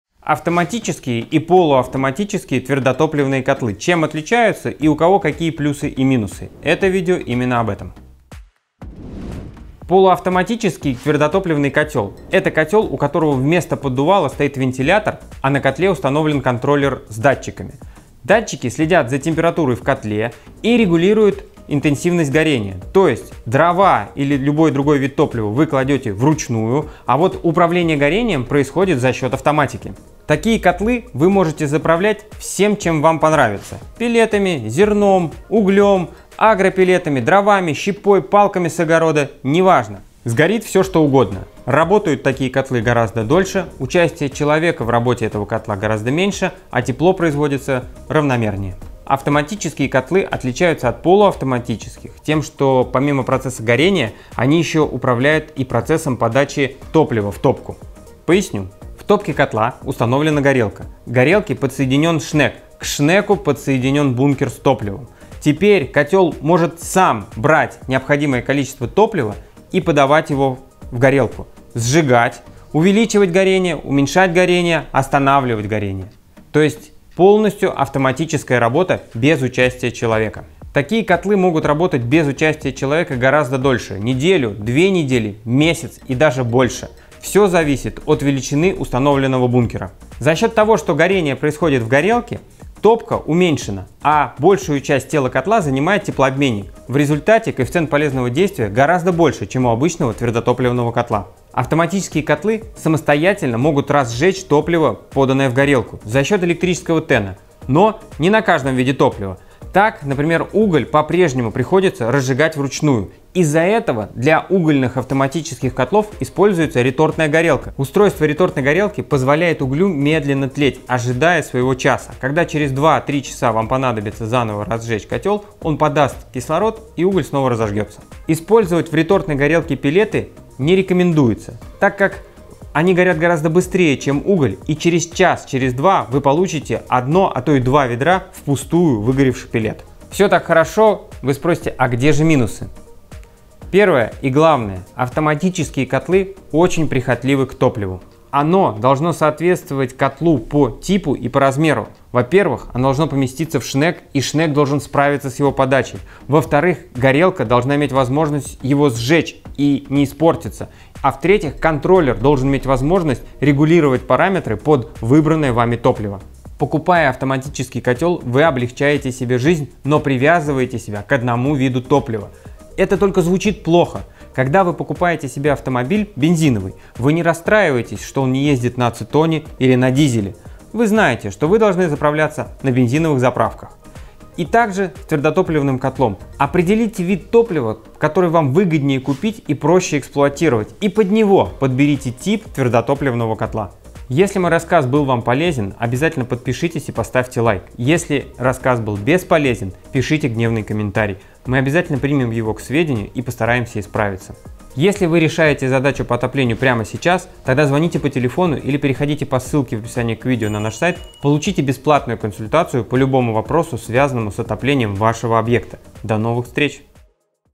Какой котел выбрать: автоматический или полуавтоматический? / В чем их отличия? ПЛЮСЫ И МИНУСЫ
Полуавтоматические твердотопливные котлы вместо поддувала имеют встроенный вентилятор, а также оснащаются контроллером с датчиками. Датчики следят за температурой и регулируют интенсивность горения, что исключает необходимость вмешательства человека. Однако дрова или уголь необходимо закладывать вручную. Зато можно загружать абсолютно любой тип топлива, хоть пеллеты, хоть зерно, хоть агропеллеты.

Автоматические твердотопливные котлы могут работать полностью без участия человека. Они сами загружают необходимое количество топлива, регулируют его подачу и температуру горения. Автономность работы может длиться от нескольких недель до пары месяцев. Все зависит от величины бункера. Если вы хотите узнать все преимущества и недостатки автоматических и полуавтоматических твердотопливных котлов, досмотрите это видео до конца!

00:00 - Анонс видео
00:09 - Что такое полуавтоматически твердотопливный котел?
00:48 - Чем автоматически котлы отличаются от полуавтоматических?
01:30 - Преимущества автоматического котла
02:19 - Недостатки автоматических котлов